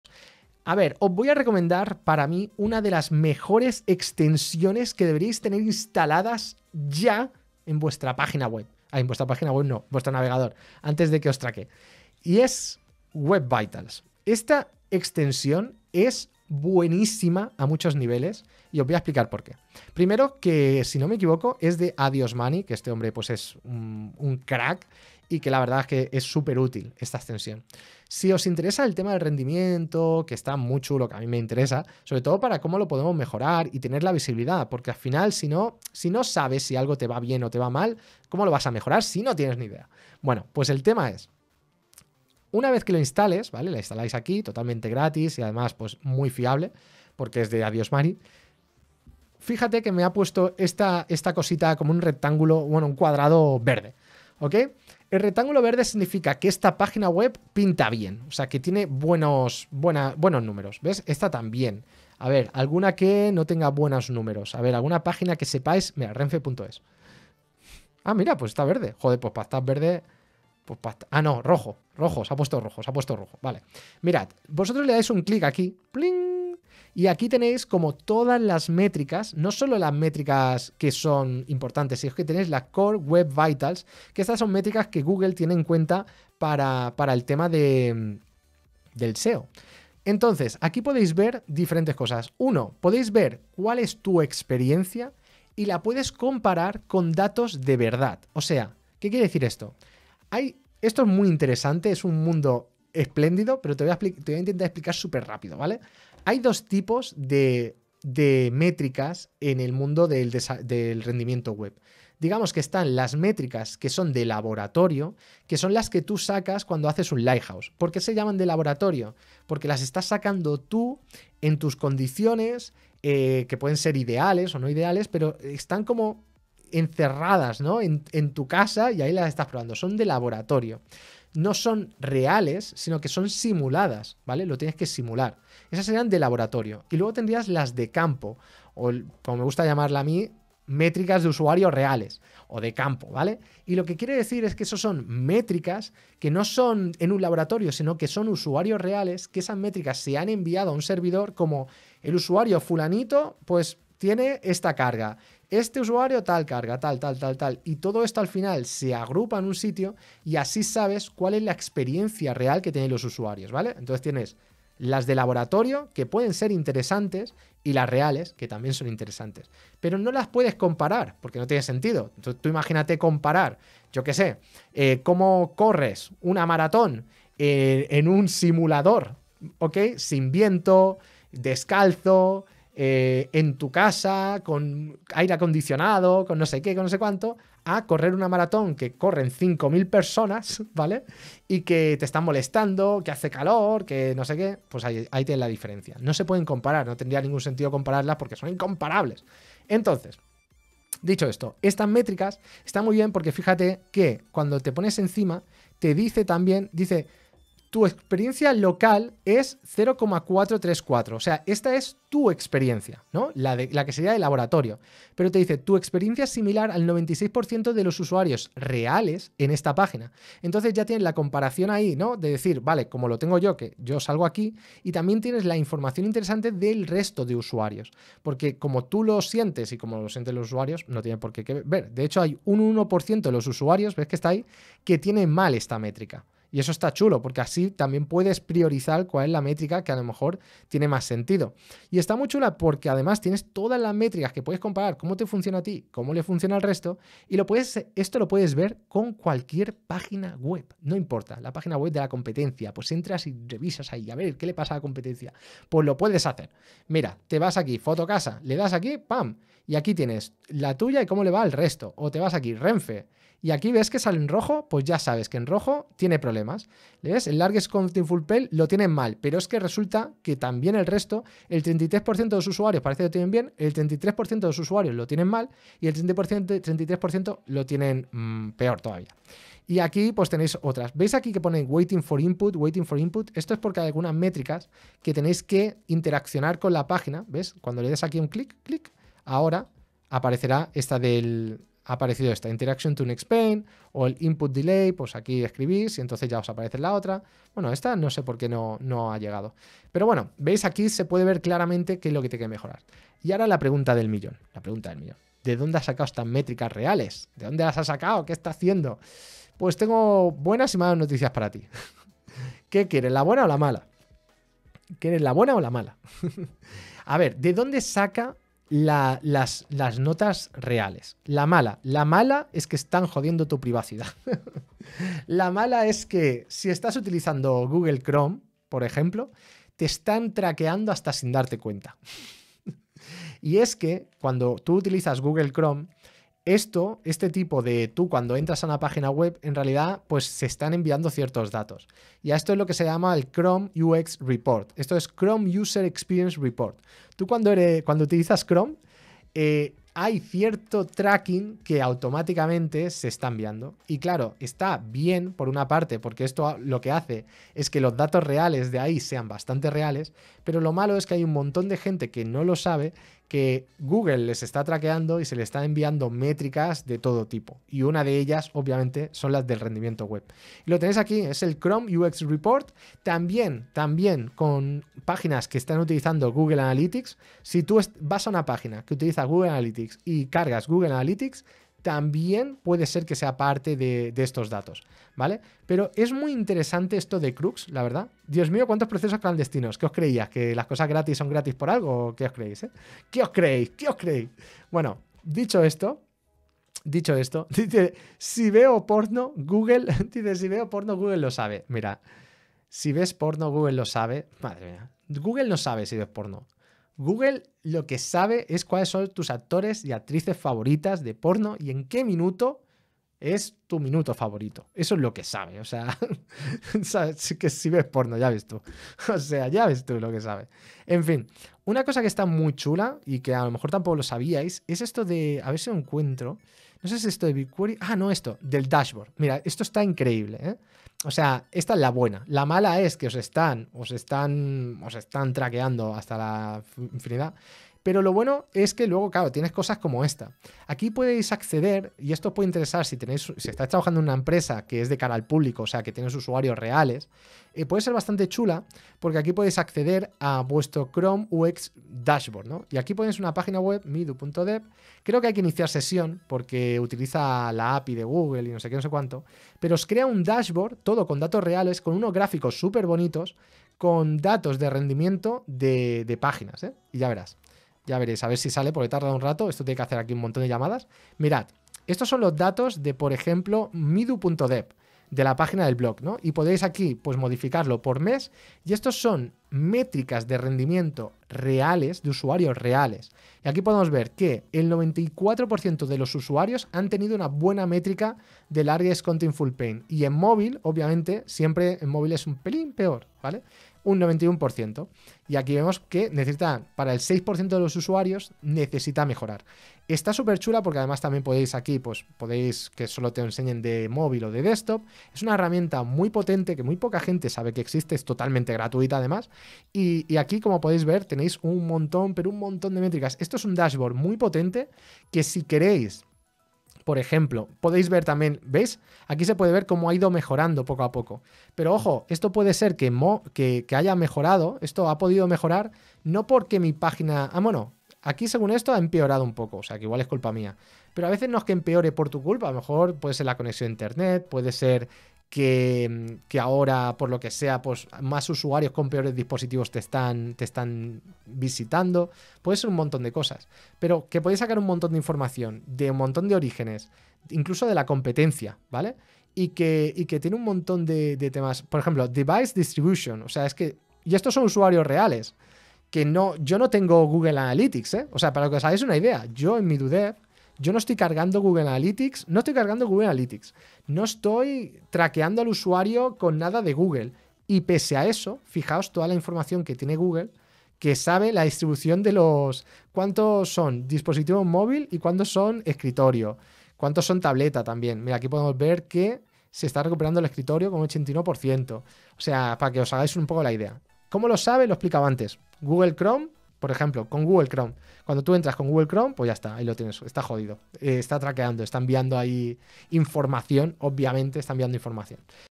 La Mejor Extensión para programadores y Empresas 📈
Descubre la mejor extensión de Google para Desarrolladores Web y Empresas. En este video, exploramos una extensión de Google que te brinda una visión detallada de cómo interactúan los usuarios con tu página web y qué dispositivos utilizan y muchas cosas más.
Con esta herramienta, podrás obtener información esencial sobre el rendimiento de cada usuario a través de la UX, permitiéndote hacer ajustes específicos y efectivos para mejorar tu página web. Ideal para cualquier profesional de desarrollo web o empresa que quiera optimizar su presencia en línea.
Lo malo es que lo gratis tiene un coste 🔥